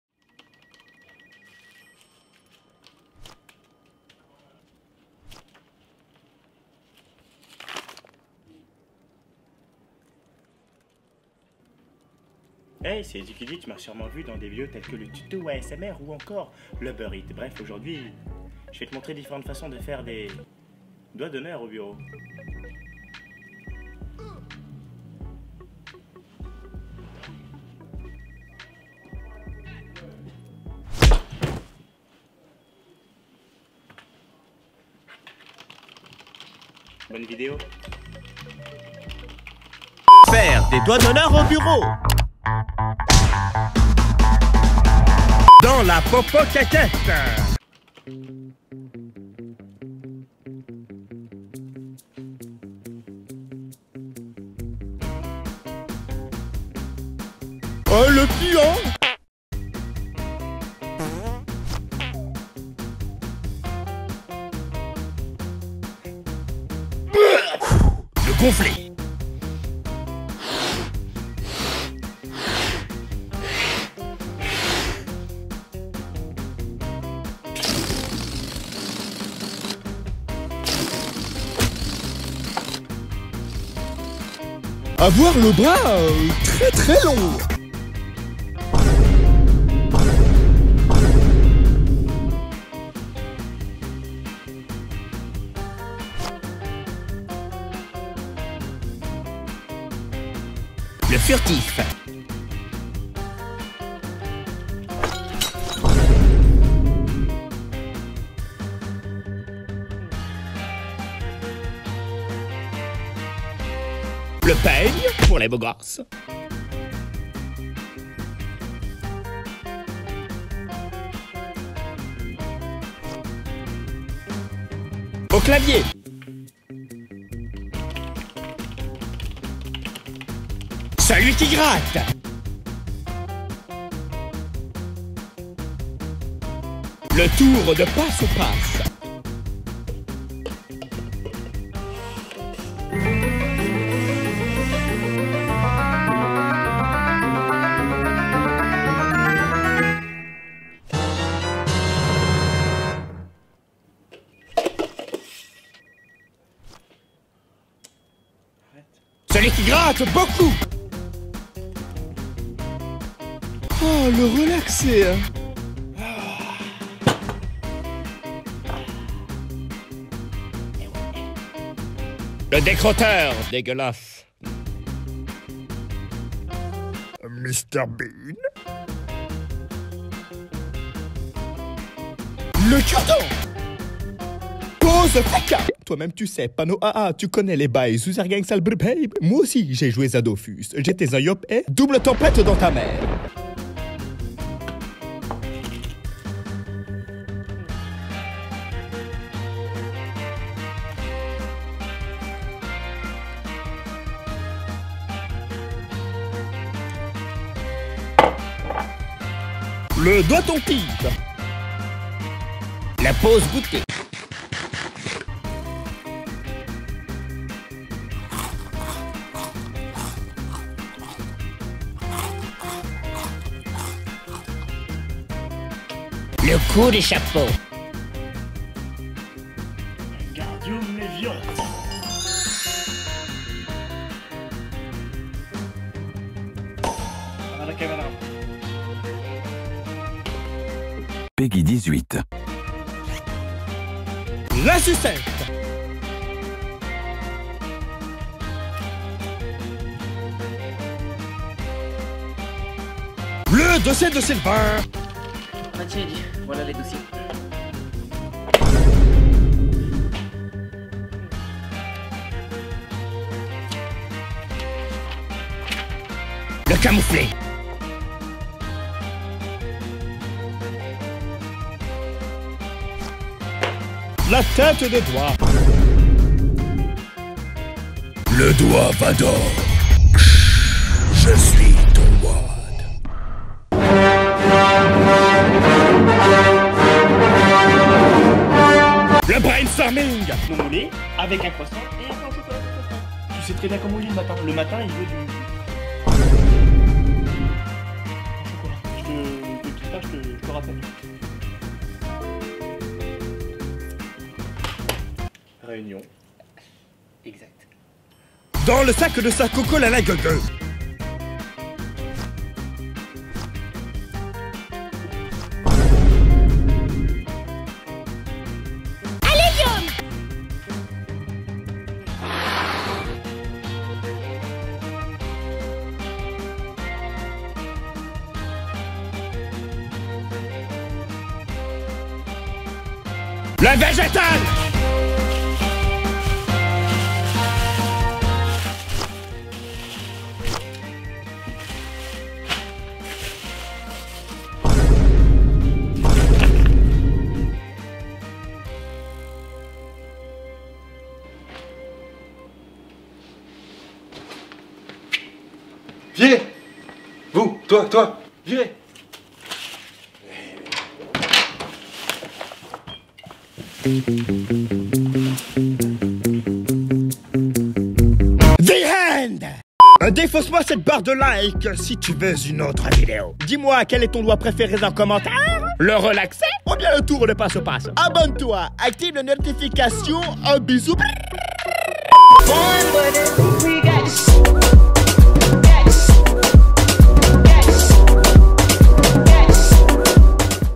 FAIRE DES DOIGTS D'HONNEUR AU BUREAU ! - EDDIE CUDI
J'adore faire des doigts d'honneur à toute les sauces !

Commente : "EN BRRR" si tu fais parti du BRRRGang